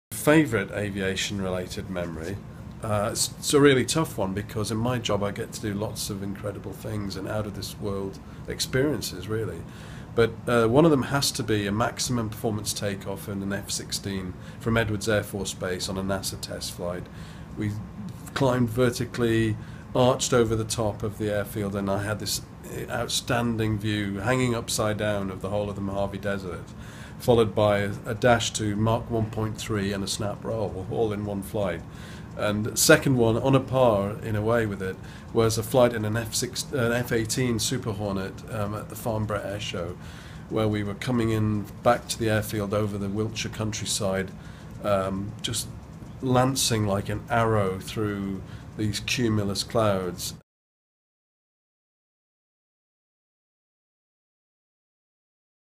Aviation Week Journalist, Guy Norris, Shares His Favorite Aviation Memory with GE Aviation
Celebrate NationalAviationDay with GE Aviation, by joining the conversation and submitting your favorite or first aviation related memory.

In 1939, President Franklin D. Roosevelt issued the first presidential proclamation designating Aug. 19 as National Aviation Day.